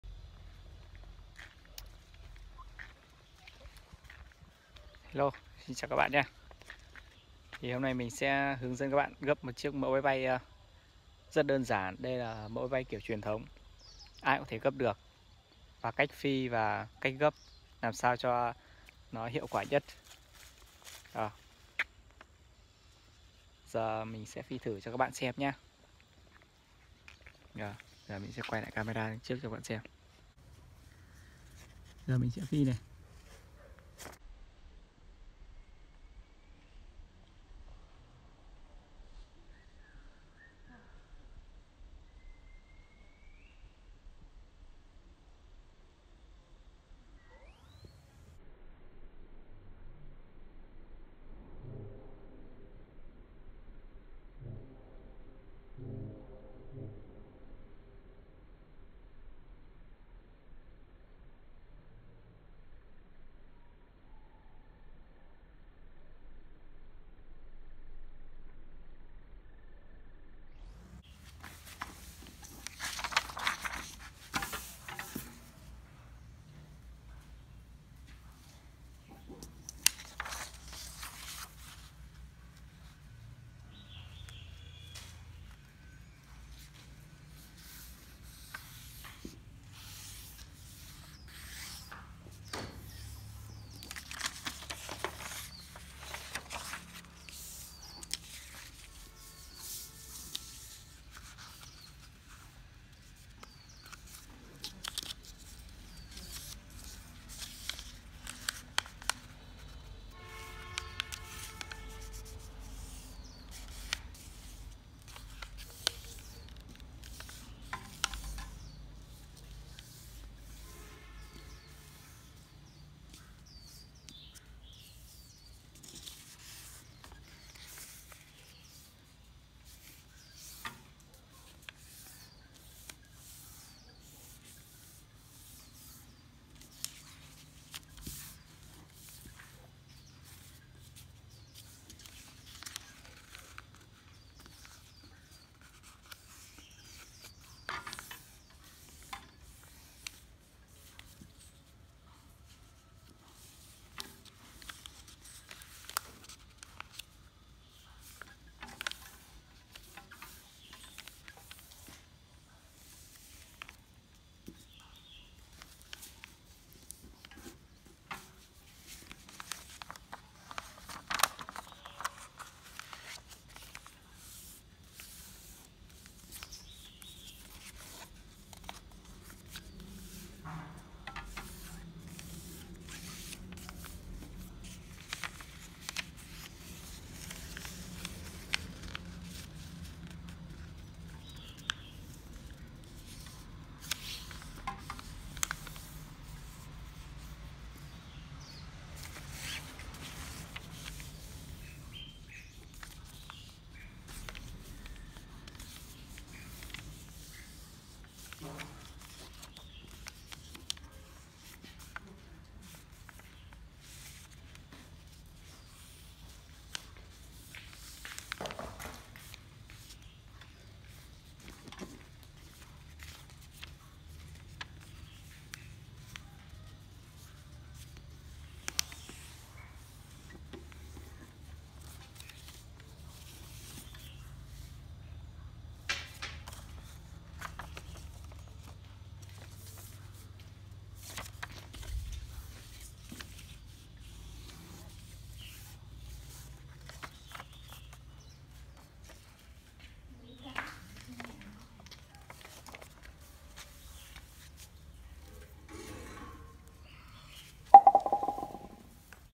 Gấp máy bay giấy đơn giản nhất
Nội dung video : Hướng dẫn các bạn gấp máy bay giấy, Cách xếp máy bay giấy theo kiểu truyền thống. Tự gấp máy bay tại nhà và trường học, làm món đồ chơi để giải trí, đây là mẫu máy bay rễ gấp và hiệu quả

Video content: Instructing you to fold paper airplanes, How to arrange paper airplanes in the traditional way. Self-folding aircraft at home and school, making toys to entertain, this is a model of folding and efficient roots